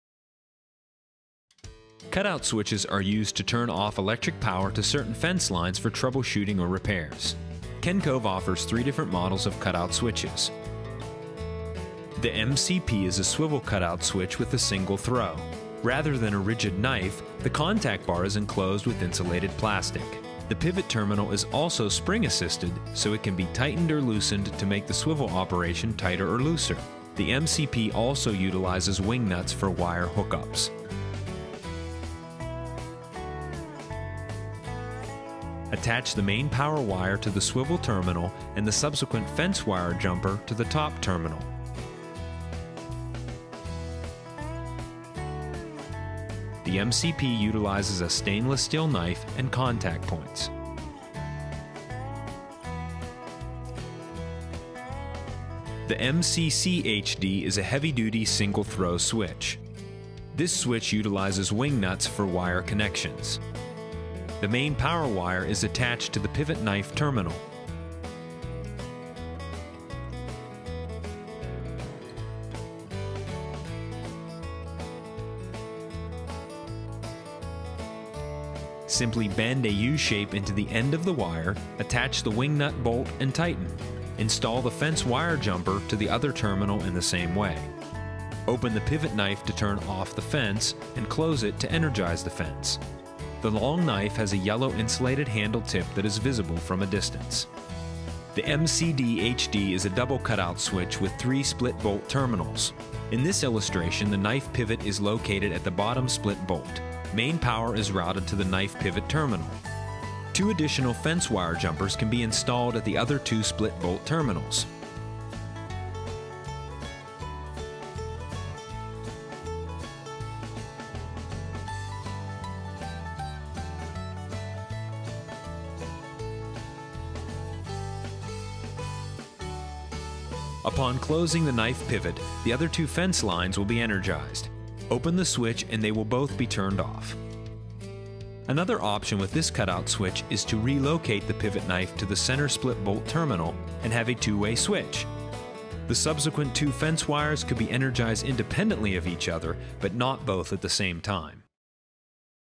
Cutout Switches
Cutout switches are used to turn off electric power to certain fence lines for trouble shooting or repairs.